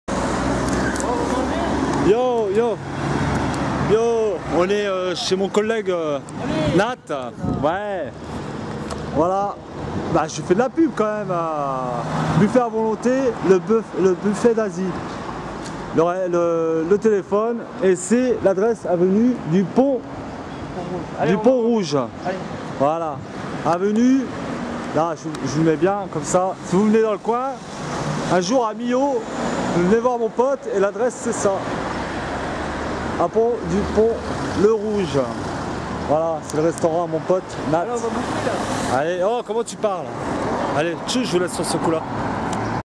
voila Millau est le buffet d 'Asie parti 3
voila le buffet d 'Asie mon ami Nhat le boss ma invite a venir voir sont restau sur Millau super . le buffet d’Asie ,buffet a volonté l adresse avenue du pont lerouge a Millau tel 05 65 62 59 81 . la bouffe et super bonne aller le voir mais amis si vous aller a Millau un jours!. et dite lui bonjours de ma par ok